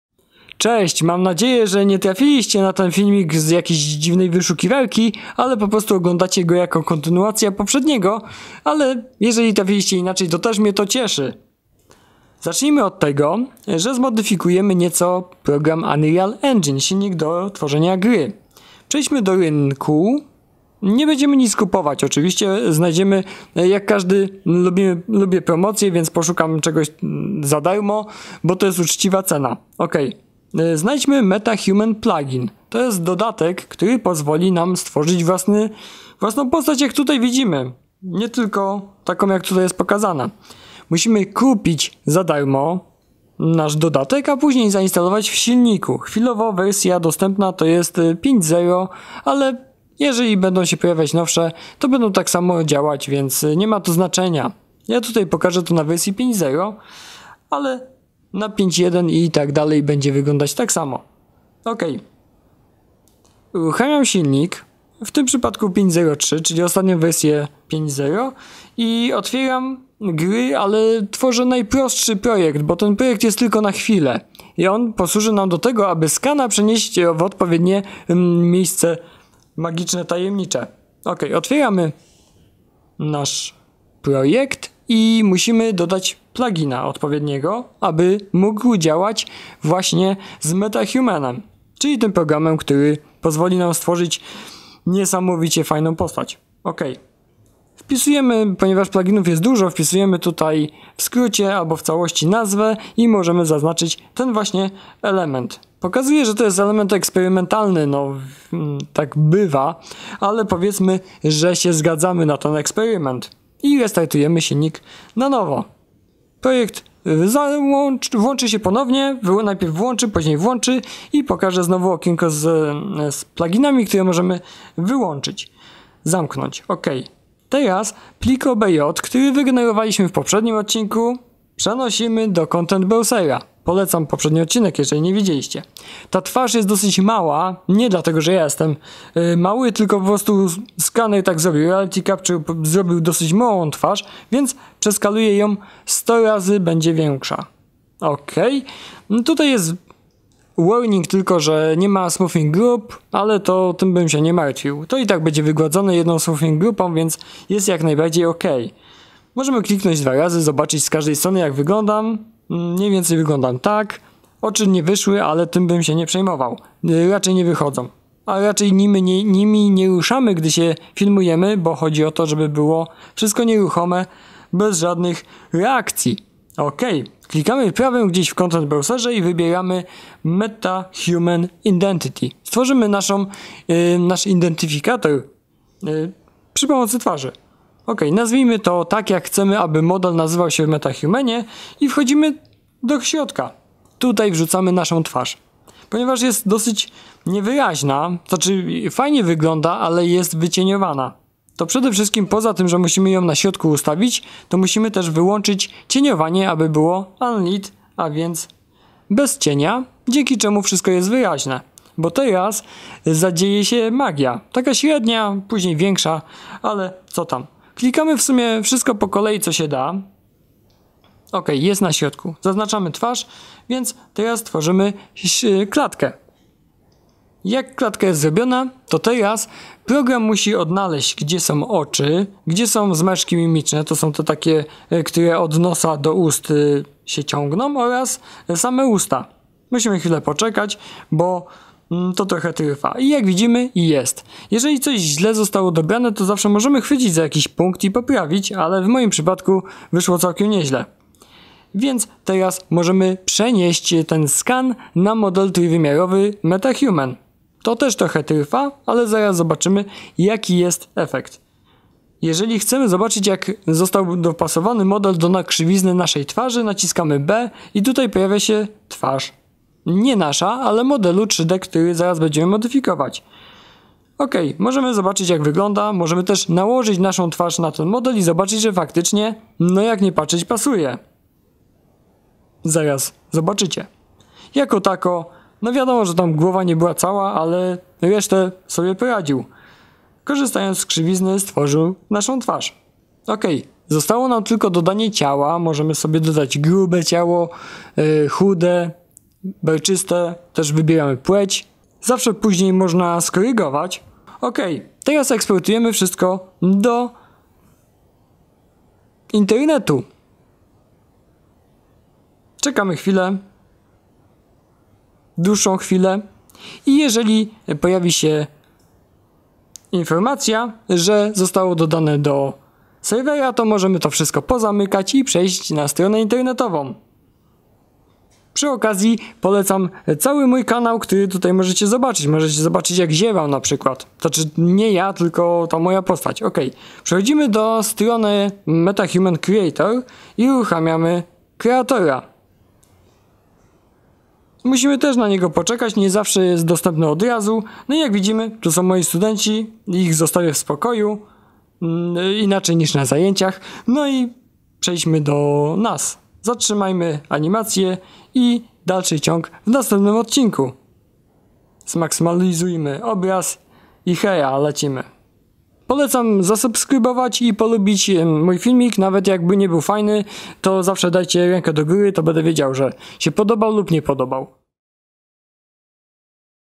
02 Unreal Engine 5 - Import skanu (from Mesh)
Wersja filmiku dla trybu skanowania from Mesh (z modelu 3D).  Posiadamy już gotowy skan twarzy, więc teraz możemy go przeanalizować.
Zrobi to plugin do Unreal Engine o nazwie MetaHuman, który dopasuje kształt skanu do modyfikowalnej postaci generycznej.

0:15 MetaHuman plugin
2:04 Import modelu
2:51 MetaHuman ID
3:46 Rozpoznawanie twarzy
5:04 Eksport do aplikacji webowej MetaHuman
5:40 MetaHuman Creator